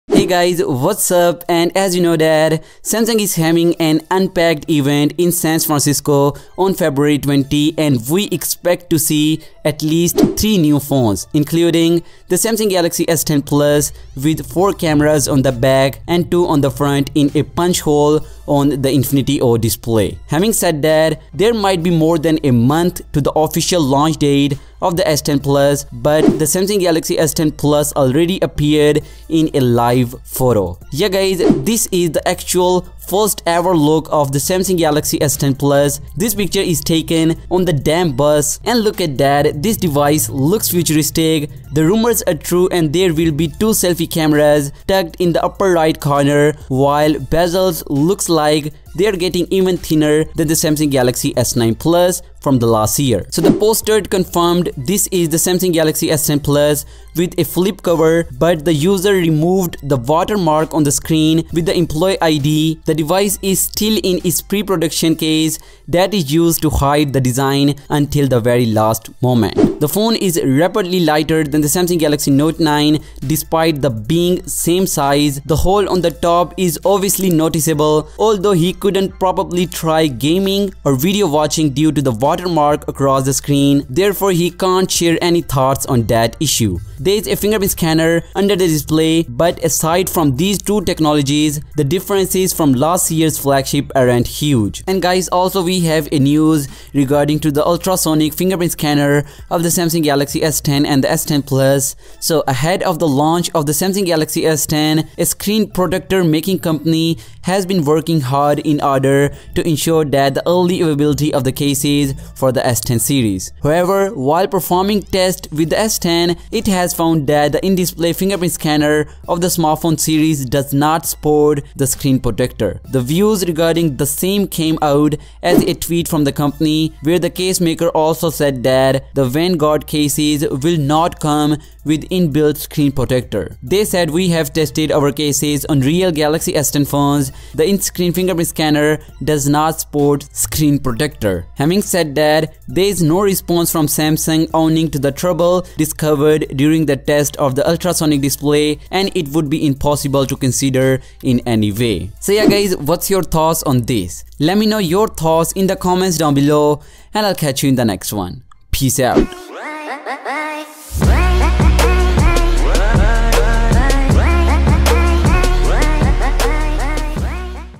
Samsung Galaxy S10 - First HANDS ON LEAK!!!
So guys Samsung Galaxy S10 Plus Hands On leak Is here , Official release date leak !!

Your thoughts on this?

PEACE !!